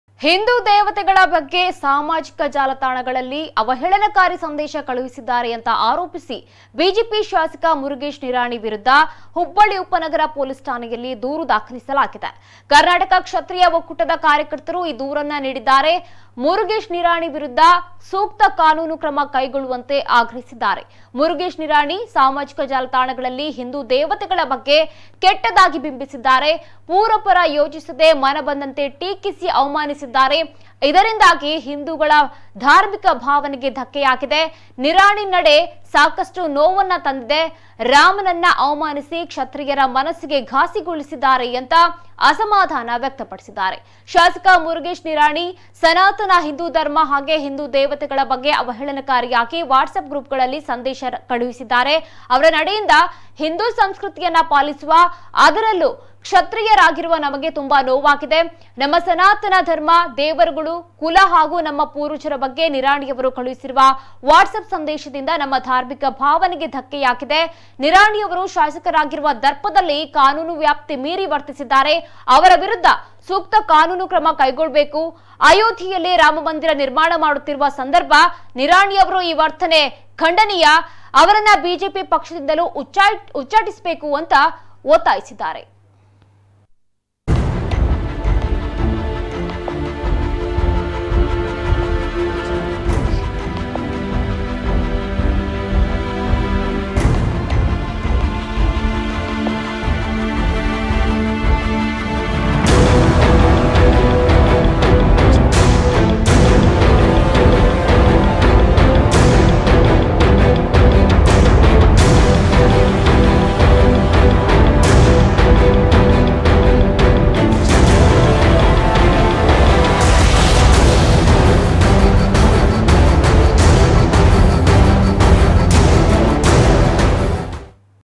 ಹಿಂದೂ ದೇವತೆಗಳ ಬಗ್ಗೆ ಅವಹೇಳನ ಬಿಜೆಪಿ ಶಾಸಕನ ವಿರುದ್ದ ದೂರು ದಾಖಲು
ಹಿಂದೂ ದೇವತೆಗಳ ಬಗ್ಗೆ ಸಾಮಾಜಿಕ ಜಾಲತಾಣಗಳಲ್ಲಿ ಅವಹೇಳನಕಾರಿ ಸಂದೇಶ ಕಳಿಸಿದ್ದಾರೆ ಎಂದು ಆರೋಪಿಸಿ ಬಿಜೆಪಿ ಶಾಸಕ ಮುರುಗೇಶ್ ನಿರಾಣಿ ವಿರುದ್ಧ ಹುಬ್ಬಳ್ಳಿ ಉಪನಗರ ಪೊಲೀಸ್ ಠಾಣೆಯಲ್ಲಿ ದೂರು ದಾಖಲಿಸಲಾಗಿದೆ.ಕರ್ನಾಟಕ ಕ್ಷತ್ರಿಯ ಒಕ್ಕೂಟದ ಕಾರ್ಯಕರ್ತರು ಈ ದೂರನ್ನು ನೀಡಿದ್ದಾರೆ. ಮುರುಗೇಶ್ ನಿರಾಣಿ ವಿರುದ್ಧ‌ ಸೂಕ್ತ ಕಾನೂನು ಕ್ರಮ ಕೈಗೊಳ್ಳುವಂತೆ ಆಗ್ರಹಿಸಿದ್ದಾರೆ. ಮುರುಗೇಶ್ ನಿರಾಣಿ ಸಾಮಾಜಿಕ ಜಾಲತಾಣಗಳಲ್ಲಿ ಹಿಂದೂ ದೇವತೆಗಳ ಬಗ್ಗೆ ಕೆಟ್ಟದಾಗಿ ಬಿಂಬಿಸಿದ್ದಾರೆ. ಪೂರ್ವಾಪರ ಯೋಚಿಸದೆ ಮನಬಂದಂತೆ ಟೀಕಿಸಿ ಅವಮಾನಿಸಿದ್ದಾರೆ.